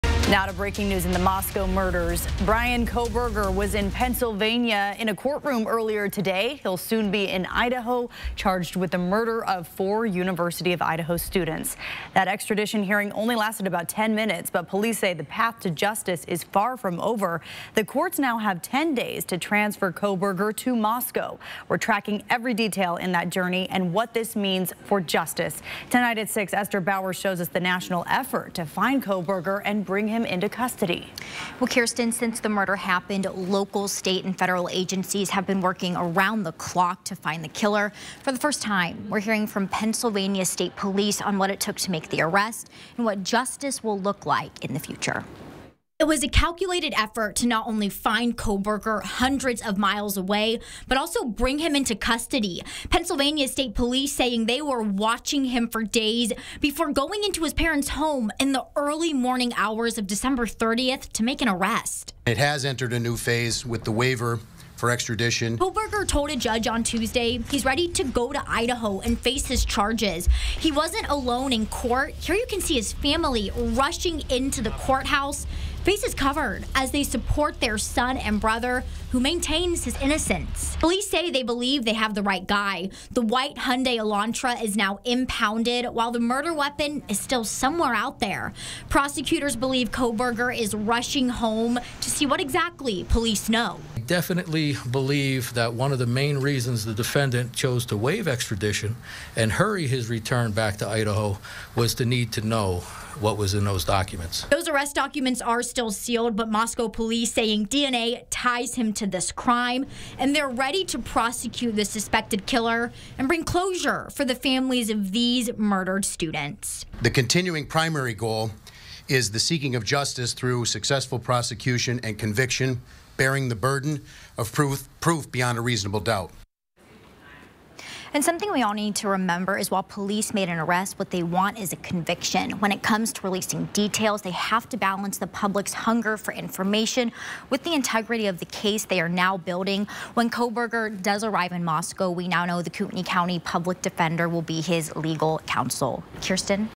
Pennsylvania State Police hold press conference on Moscow murder suspect’s arrest
MONROE COUNTY, PA — Officials in Pennsylvania held a press conference on Tuesday regarding their role in the arrest of Idaho murder suspect, Bryan Kohberger.

The 28-year-old was taken into custody in Monroe County on Friday, December 30.
Beginning the press event was Colonel Robert Evanchick, Commissioner of the Pennsylvania State Police. Col. Evanchick thanked the collaboration of law enforcement efforts that led to this arrest, including Moscow Police and the FBI. Evanchick also commended Troop N and the Special Emergency Response Team that conducted the arrest.

Major Christopher Paris, Area III Commander, said the FBI had reached out several days prior to the arrest for PSP’s assistance in surveillance of the Chestnuthill Township home Kohberger was located at.

Monroe County Assistant District Attorney Mike Moncuso says enough probable cause was obtained to execute three separate search warrants on Kohberger, the residence, and a white Hyundai Elantra. Moncuso noted since the warrants were executed at night, additional probable cause was required. The probable cause affidavits are sealed from the public until handed to Kohberger in person, who waived his extradition rights streamlining his return to Idaho. Moncuso speculated Kohberger waived extradition in order to see what was in the affidavits sooner.

Kohberger is required to go to Idaho within 10 days, but the method and exact date will not be revealed. He faces four counts of first-degree murder and one count of felony burglary.